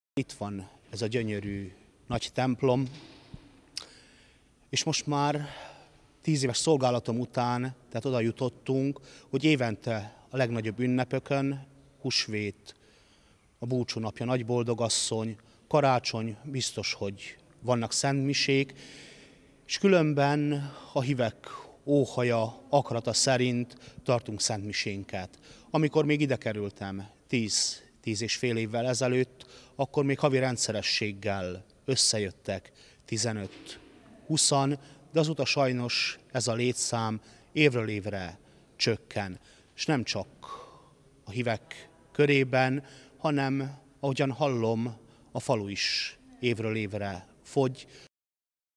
2022.04.17. - Az ünnepi mise erejéig Módoson is megnyílt a templom ajtaja
Jézus Krisztus feltámadását ünnepeli a bánáti katolikus hívőközösség is. Ezen a napon megnyílik sok olyan templom ajtaja, ahol évente csak néhányszor mutatnak be szentmisét. Így volt ez ma a jobb időket is megélt módosi Nagyboldogasszony védelmébe ajánlott katolikus templomban is.